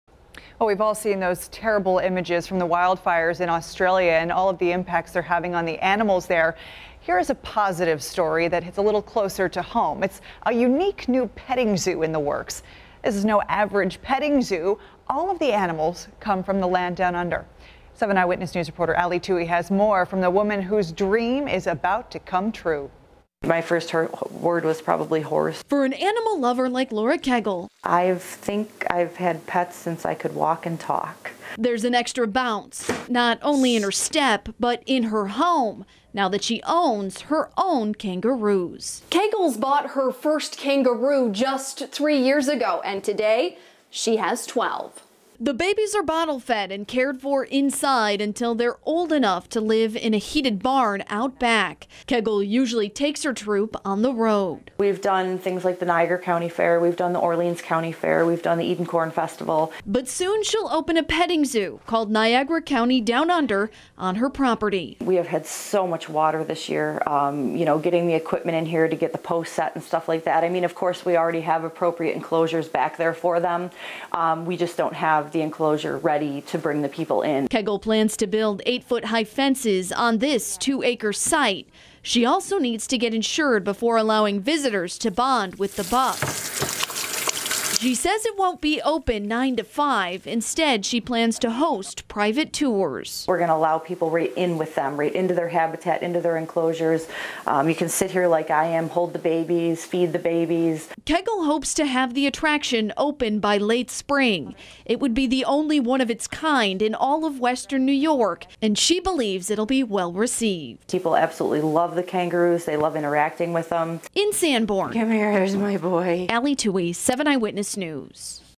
Kangaroo petting zoo proposed in Sanborn
A Sanborn woman is trying to open a unique petting zoo in Niagara County.